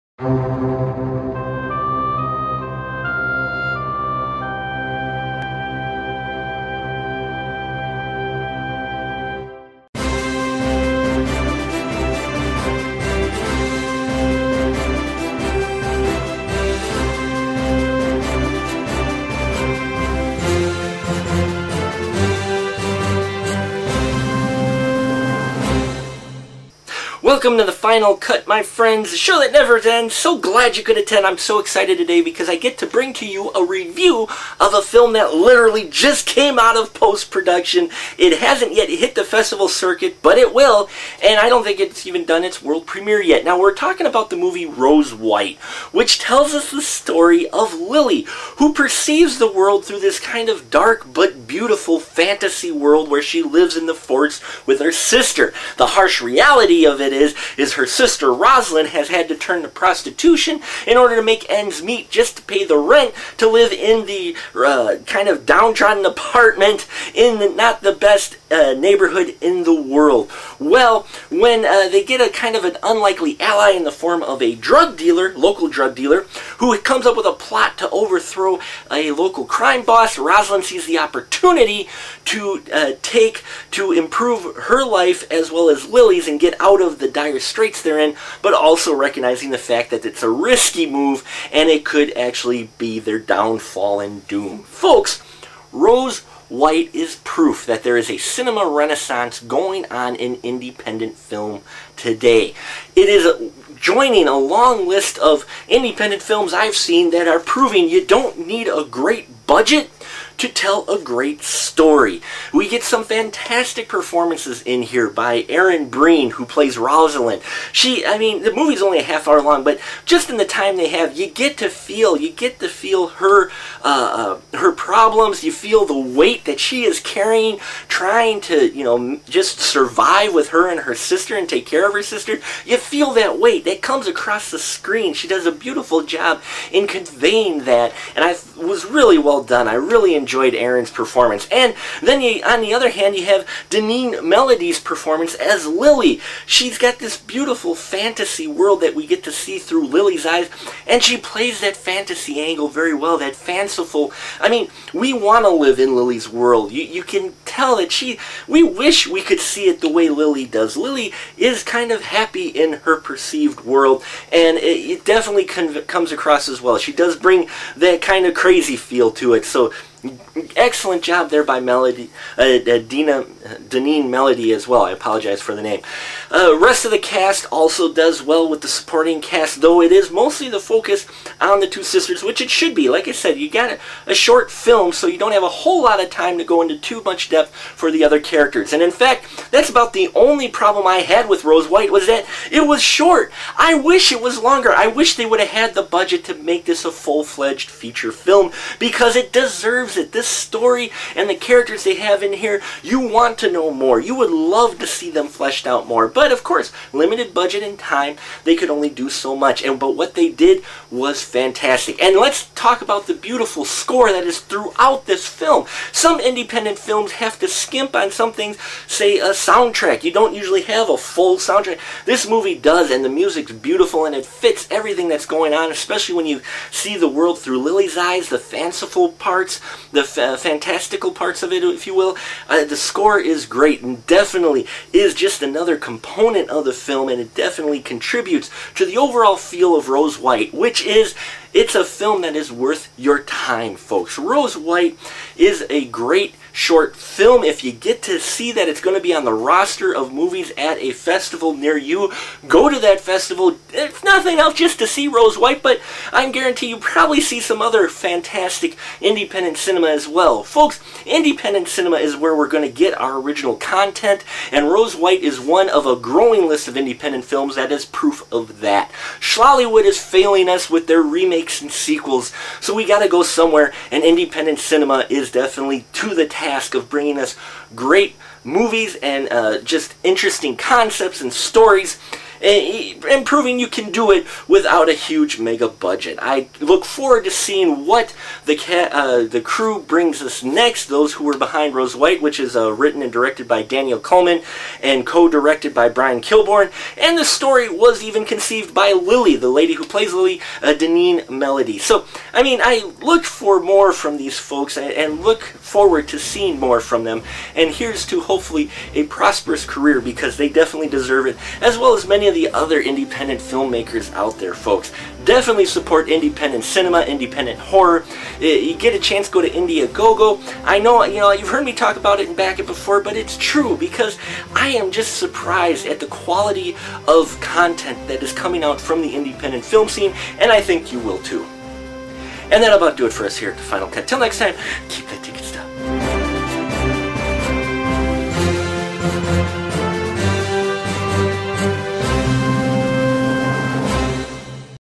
Rose White review on the Final Cut
Today we stop from watching mainstream movies to smell the roses. Rose White to be exact, a new independent short film so fresh it hasn't hit the festival scene, yet. Come inside and get our take on this latest from Breakwall Pictures.

Remember to support independent cinema my fellow moviemaniacs